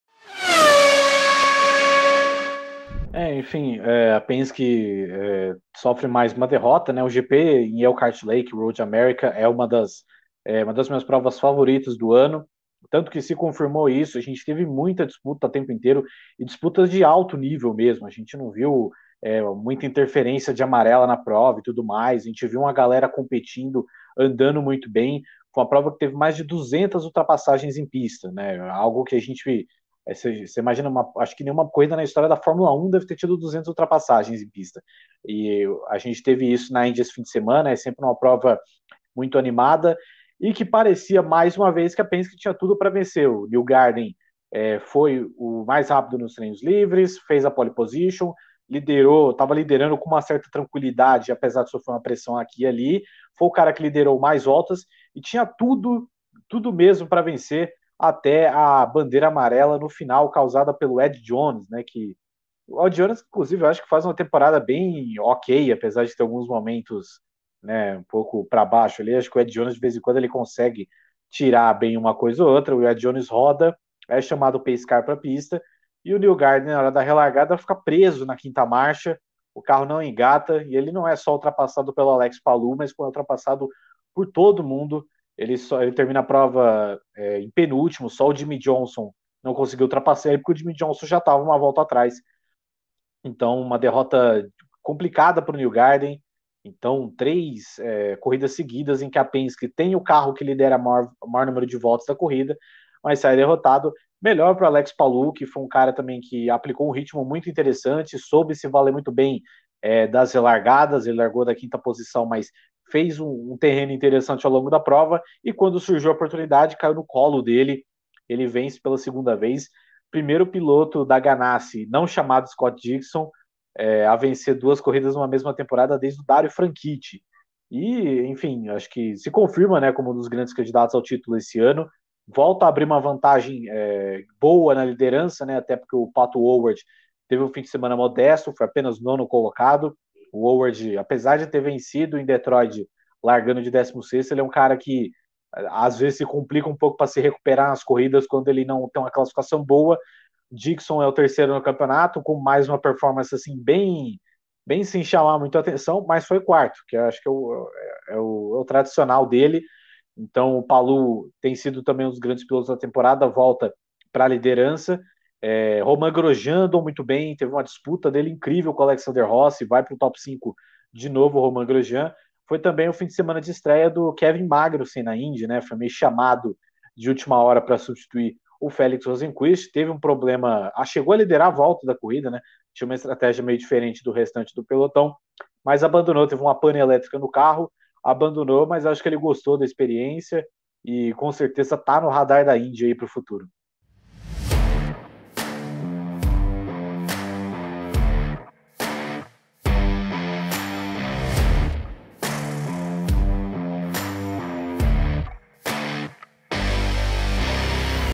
ROAD AMERICA CONFIRMA 2021 DE ALTÍSSIMO NÍVEL DA INDY, DIZ GABRIEL CARVALHO
No Paddock Plus, Gabriel Carvalho, Gabriel Curty e Vitor Fazio analisaram o que de melhor aconteceu no GP de Elkhart Lake da Indy, disputado em Road America, e com vitória de Álex Palou.